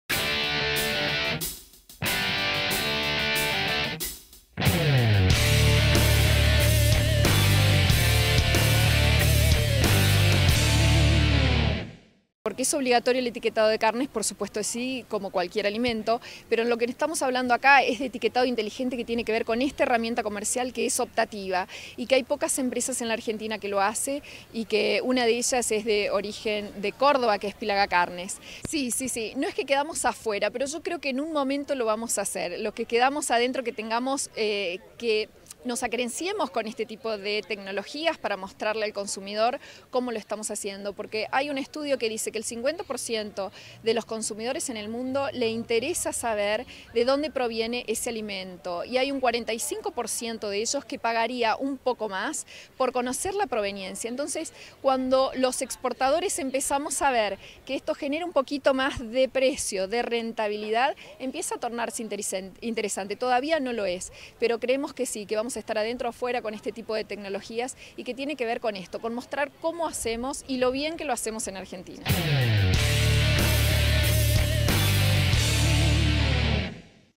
3390 - Trazabilidad y tecnologías en etiquetado de Carne, Paola Carreño
3390 - Trazabilidad y tecnologías en etiquetado de Carne, Paola Carreño, Especialista en Agronegocios